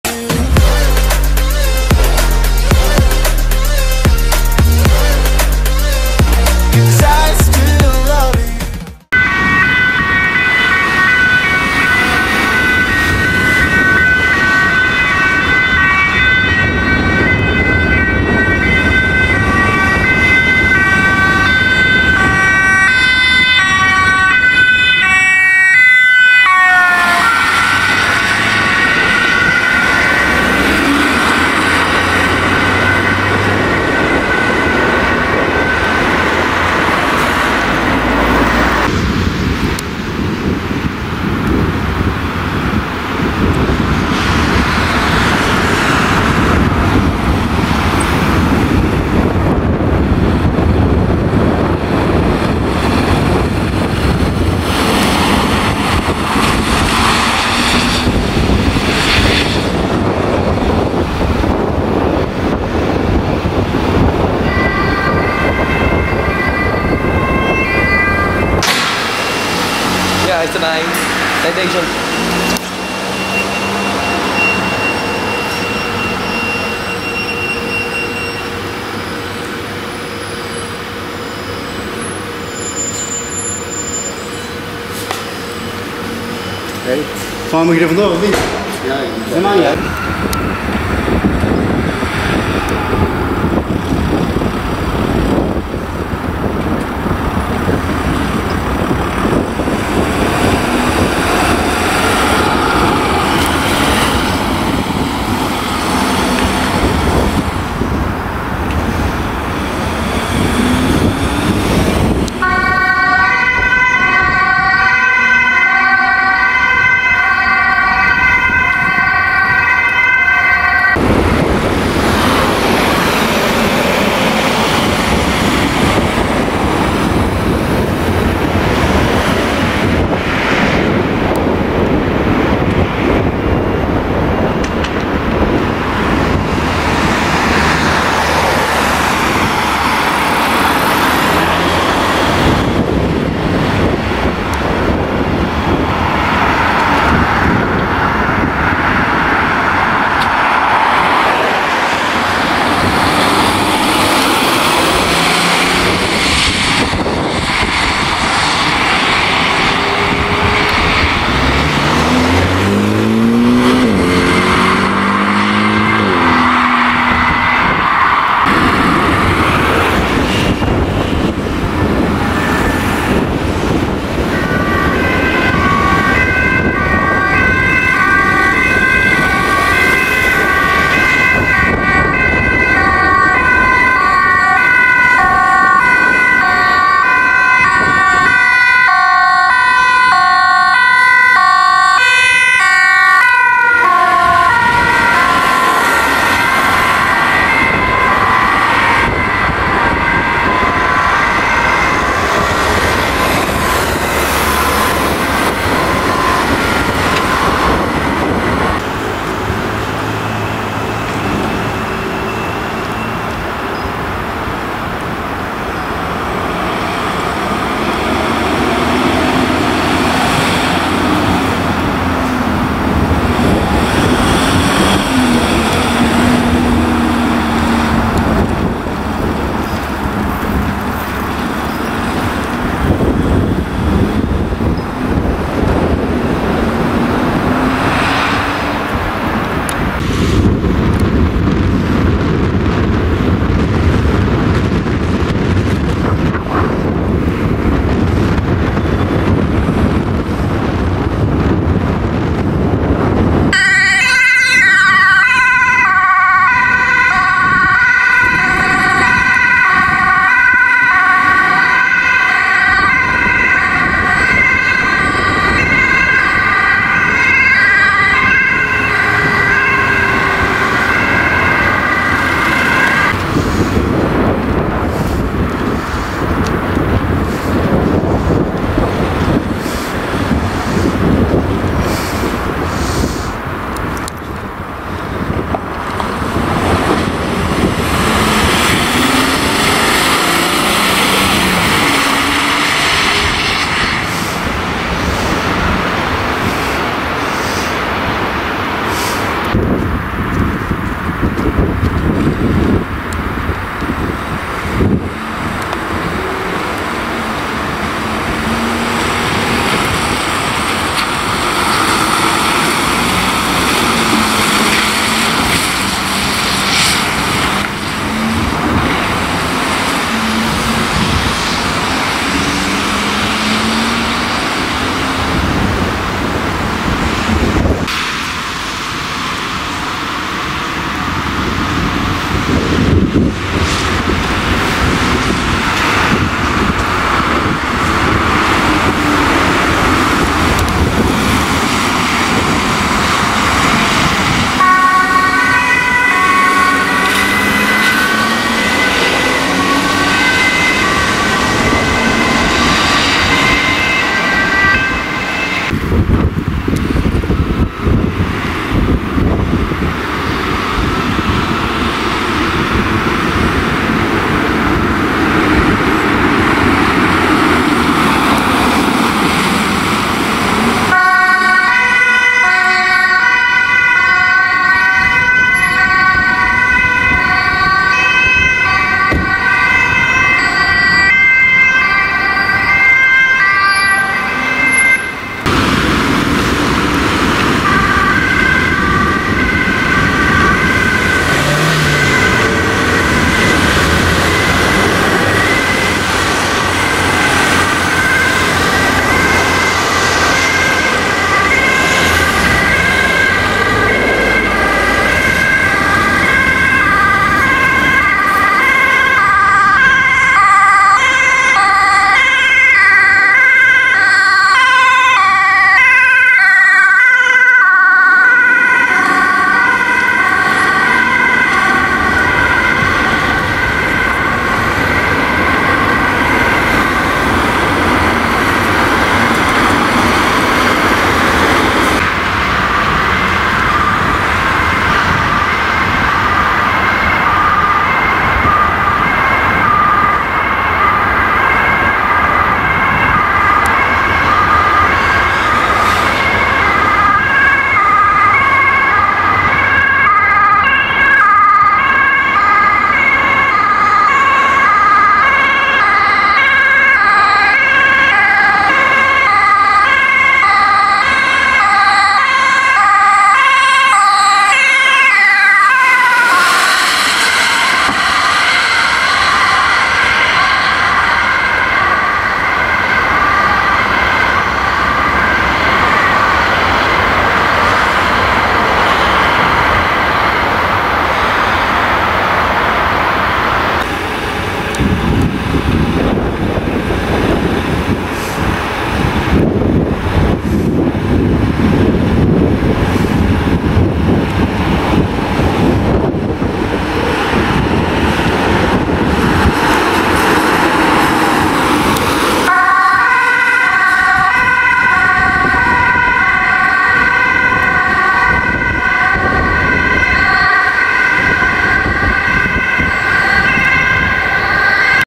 [Vanuit de Post] Verschillende ambulances met spoed van en naar meldingen in Amsterdam!
Gefilmd door: Daïn
Gefilmd met: Panasonic HC V770

U ziet:
0:00 - Intro
0:09 - Ambulance 13-179 | Met spoed naar het AMC
0:39 - Ambulance 13-104
1:08 - [Vanuit de Post] Specialty Care Ambulance 13-162
1:30 - Ambulance 13-103
2:01 - Ambulance 13-104
2:48 - Rapid Responder 13-342
3:28 - Ambulance 13-101
4:00 - Specialty Care Ambulance 13-162
4:33 - Ambulance 13-150
4:49 - Ambulance 13-105
5:28 - Ambulance 13-102
5:56 - Ambulance 13-103
6:25 - Specialty Care Ambulance 13-161
7:12 - Specialty Care Ambulance 13-164
8:04 - Specialty Care Ambulance 13-163

Bedankt voor het kijken! ! Deze video mag niet worden gebruikt zonder toestemming van de eigenaar!